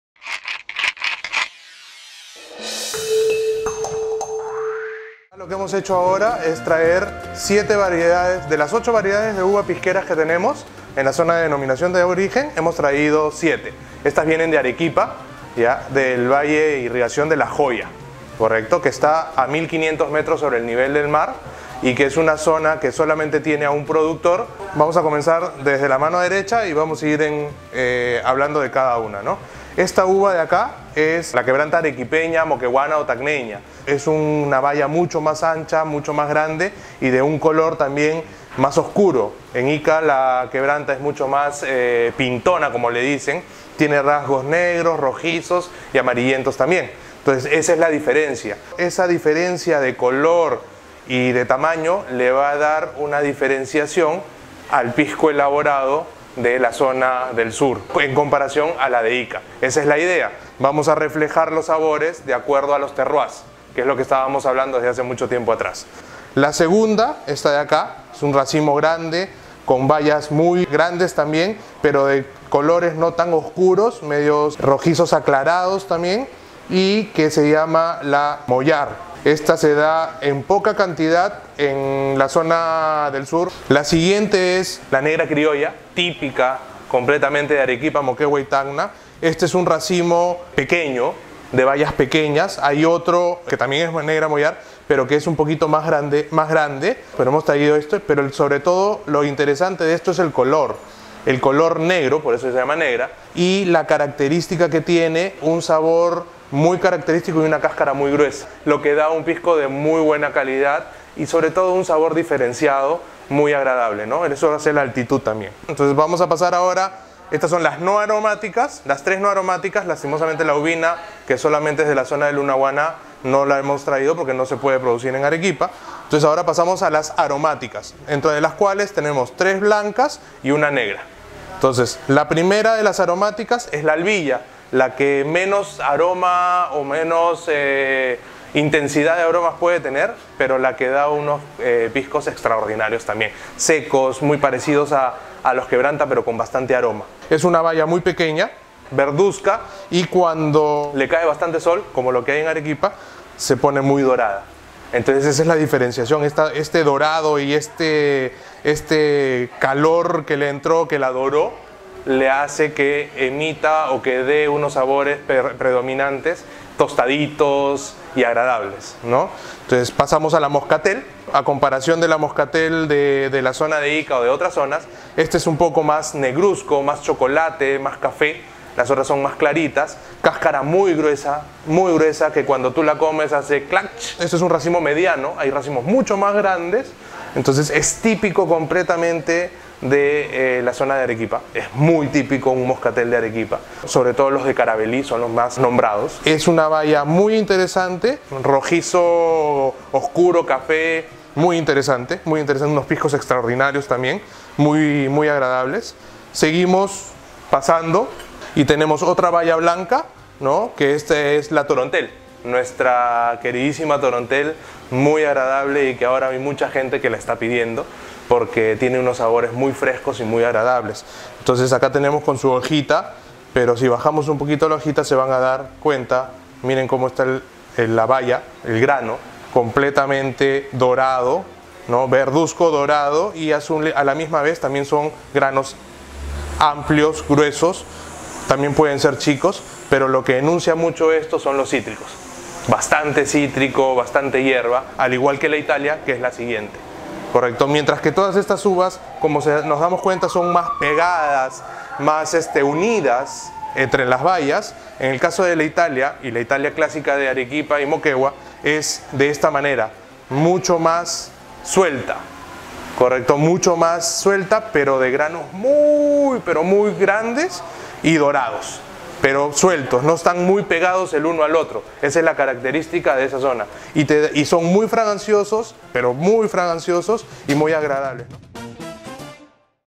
Conoce siete de las ocho uvas pisqueras del Perú
Alrededor de los comienzos de marzo los valles dedicados a la vitivinicultura en el Perú comienzan la etapa de la vendimia, que es cuando se cosecha la uva para luego convertirla en vino, y en este caso luego del vino, a través de la destilación, viene el pisco.

Por esta ocasión especial del año, Ricardo Carpio, dueño del Pisco Bar, invitó a Gestion.pe para mostrar cómo son las cepas pisqueras provenientes de la zona de La Joya, de Arequipa.

Carpio puso en la mesa siete de las cepas pisqueras, las cuales pertenecen a la familia de las uvas vitis vinífera. La octava faltante es la uvina, la cual es una uva de mesa y la producción de pisco con ella solo está permitida en el valle de Lunahuaná, en Cañete.

Video por Paulo Rivas Peña